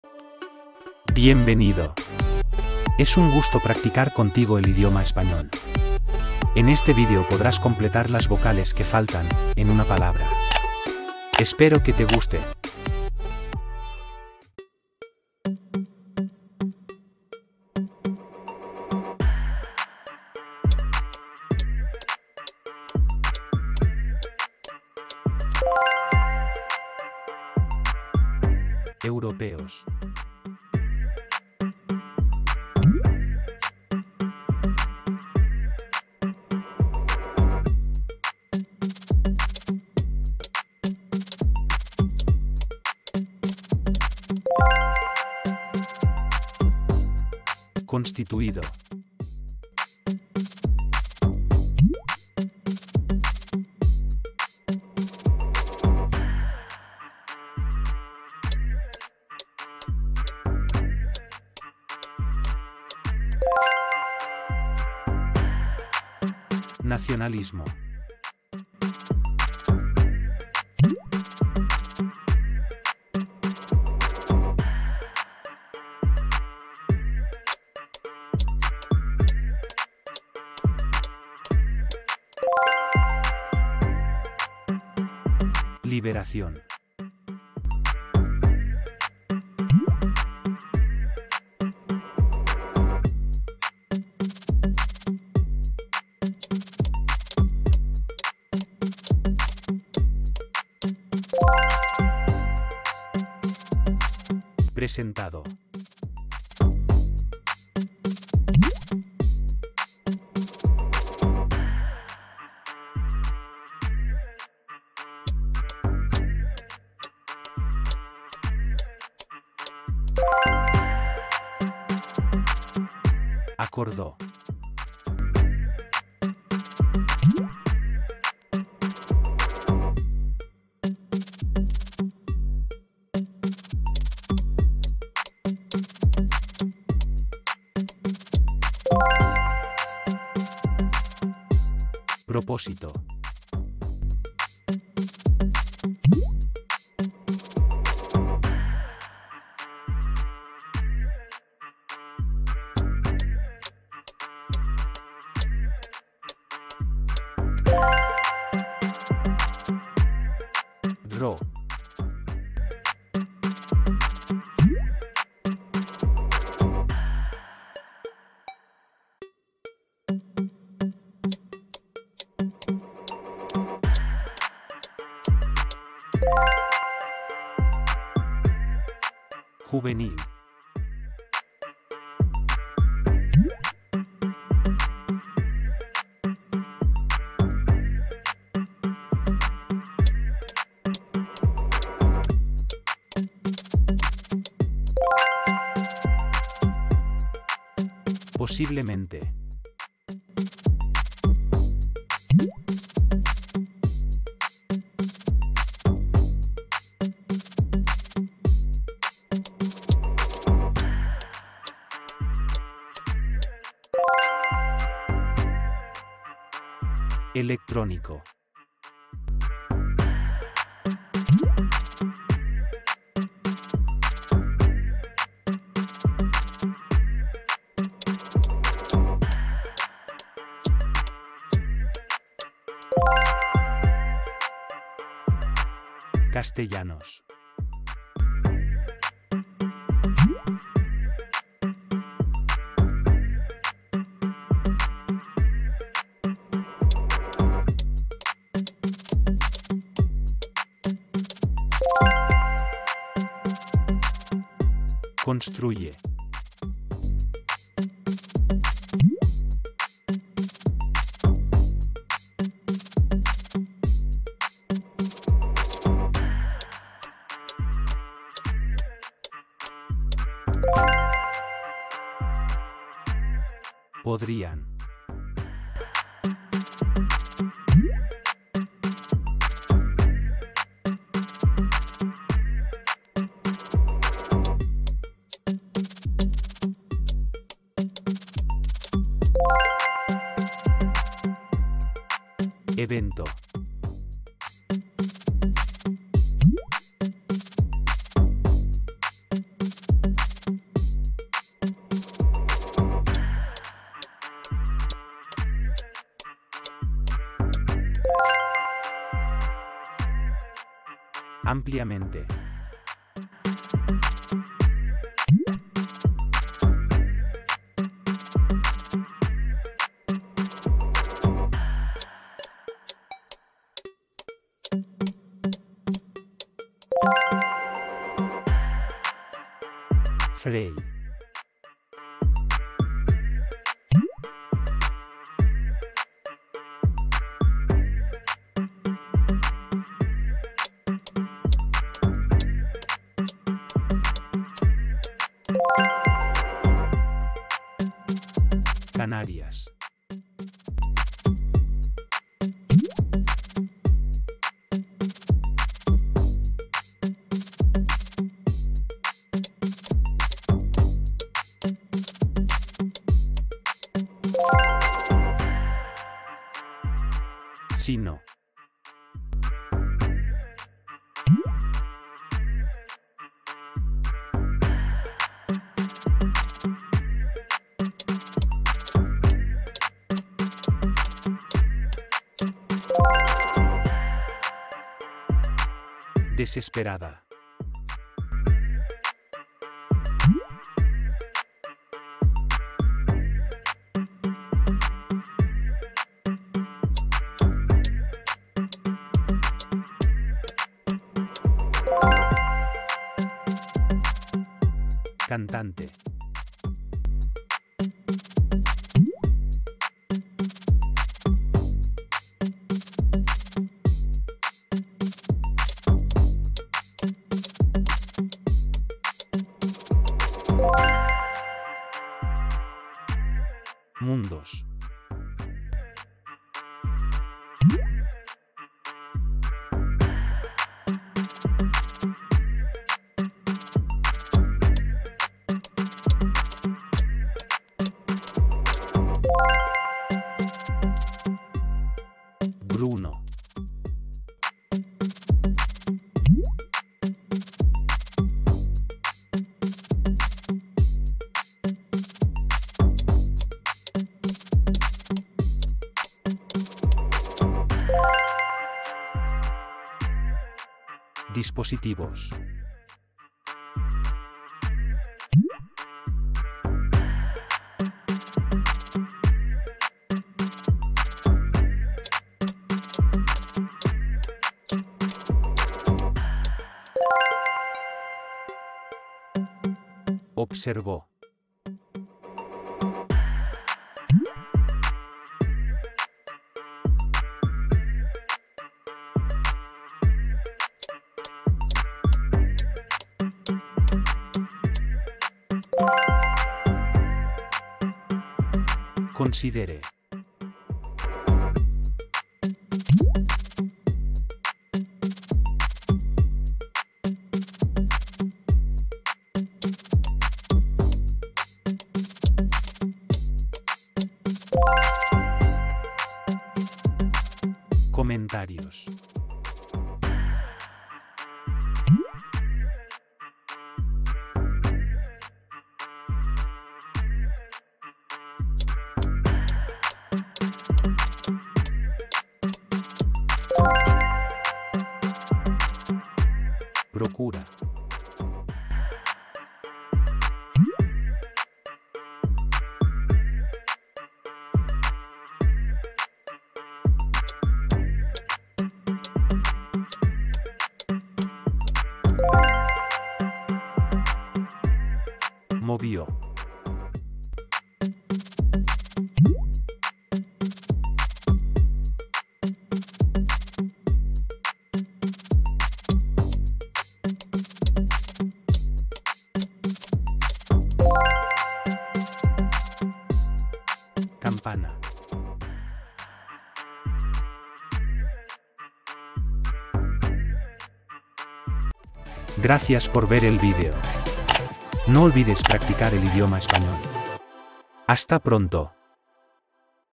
APRENDE ESPAÑOL. Completa las vocales faltantes, en una palabra. Parte 273
En este video podrás completar con vocales una palabra. Esto te permitirá mejorar tu vocabulario en Español. En un juego de completar con vocales en una palabra y tienes 10 segundos para acertar la palabra correcta.
Este método gratis de aprender Español de manera divertida, te permite expandir tus conocimientos del idioma español. Podrás practicar el escucha y la escritura de este idioma y así comunicarte con hablantes nativos del idioma Español.